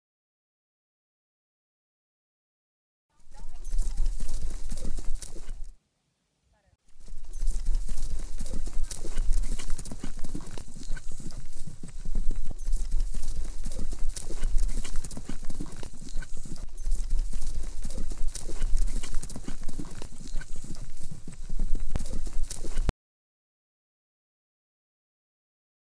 John Charles Dickson Rides a Horse - 01/21/15
Actor/Model John Charles Dickson rides a horse.